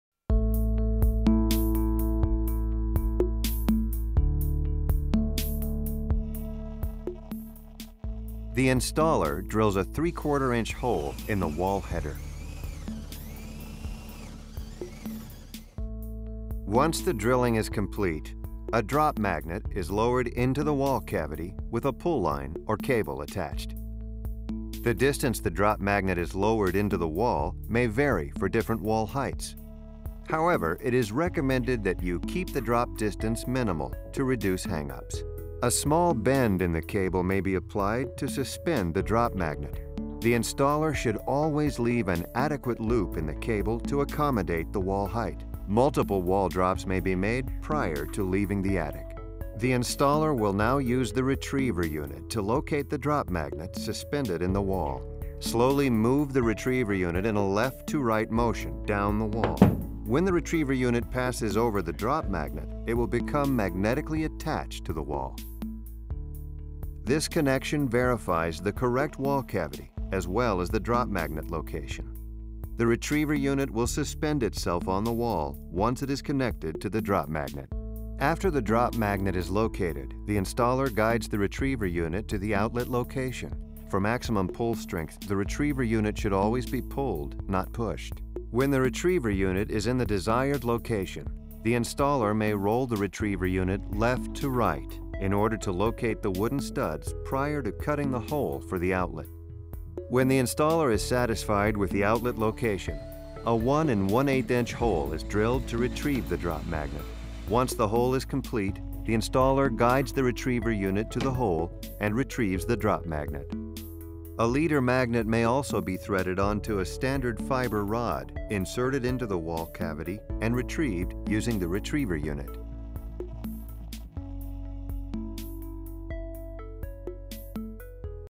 MAGNEPULL XP: How to Fish a Hollow Wall
- This incredible tool will give you a return on your investment the first time you use it! The Magnepull cuts wire fishing time by up to 70%! Eliminate multiple trips in and out of attics and crawl spaces. This amazing tool magnetically couples the cable-pulling device through drywall and allows technicians to have complete control. The system was developed to work with metal studs, wood studs, insulated walls, and even carpet! Great for Alarm, Cable, Satellite, Home Theater, Electrical, CCTV, Access Control, DataComm, and any other type of installation where wire fishing is necessary.